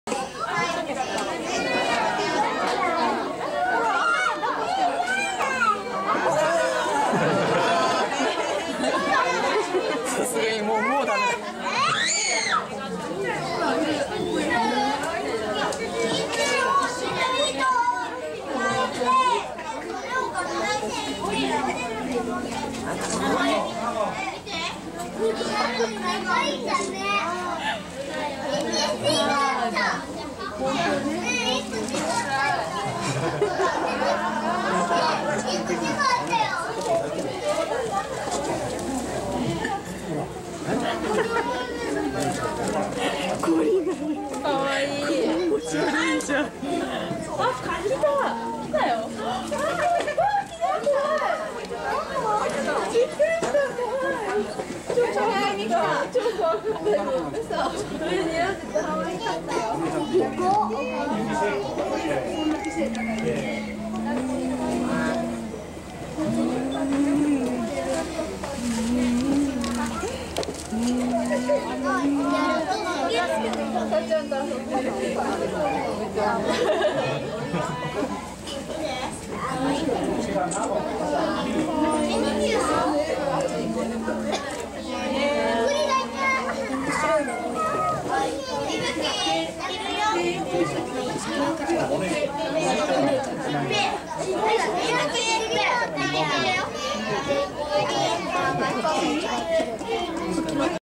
しつこいコモモちゃん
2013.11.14 上野動物園ニシローランドゴリラのコモモちゃん。とうとうお母さんに妹モモカちゃんを取り上げられてしまいましたが、まだまだあやしたくて諦めがつきません。再び誘拐を試みますが...